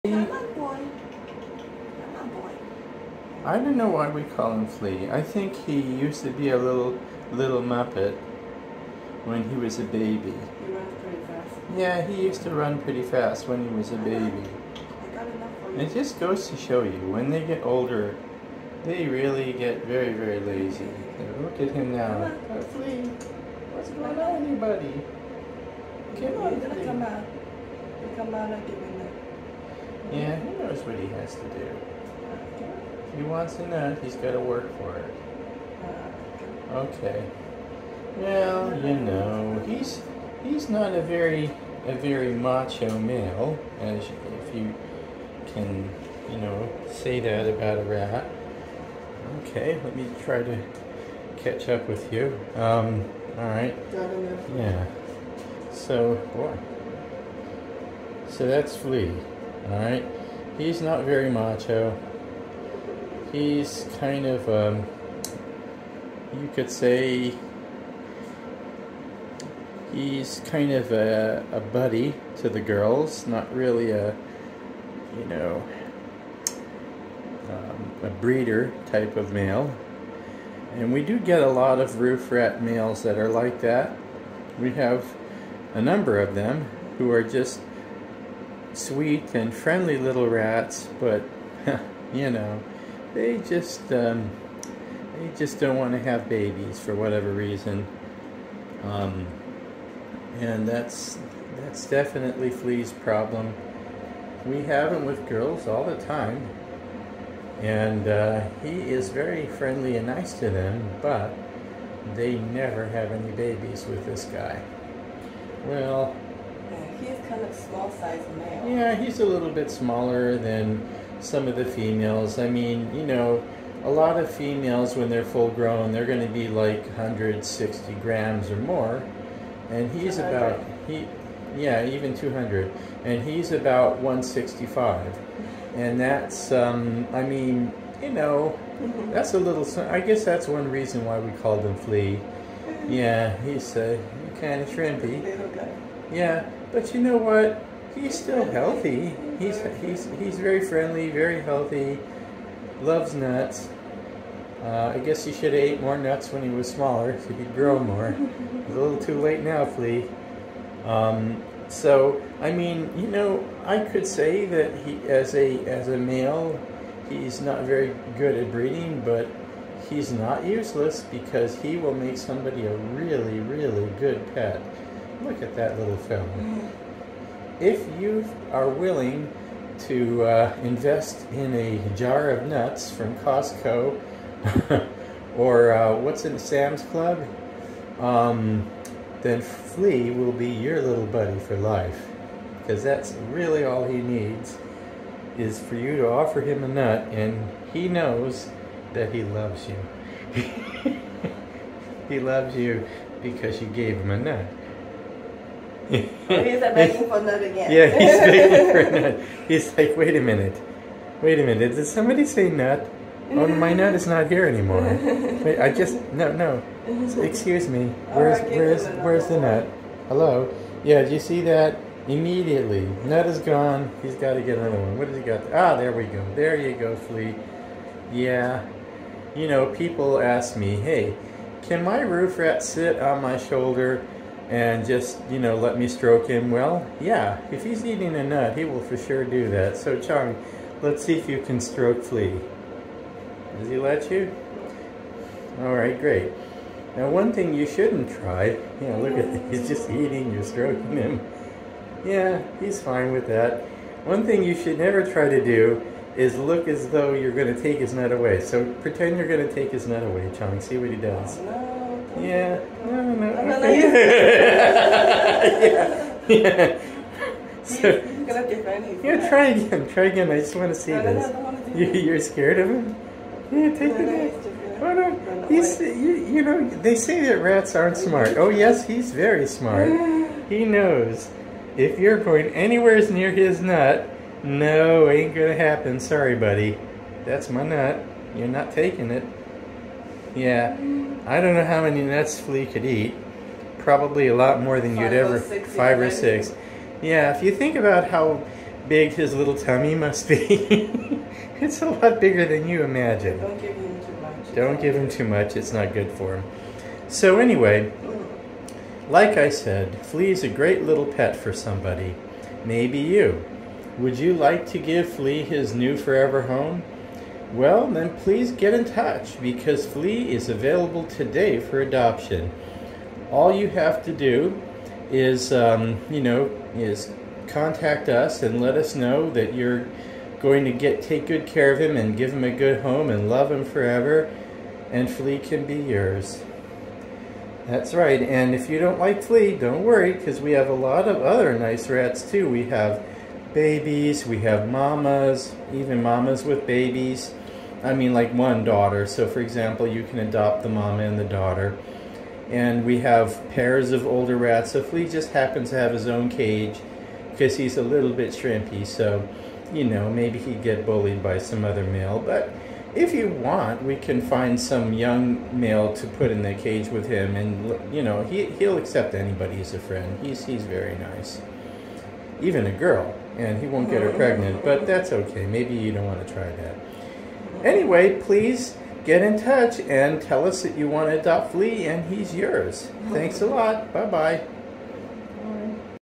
Free rat adoption - Friendly male Roof Rat available in Kissimmee Florida
Flea is a super friendly, gentle and cooperative adult male Roof Rat.  He has no known health issues and loves eating almonds.

He is available for adoption today absolutely free of charge.

If you are interested in Flea, any of our rats, or any future litters we might have, please get in touch

Pet adoptions are always 100% free.  And don't forget to "Like" this video.  Thanks! :-)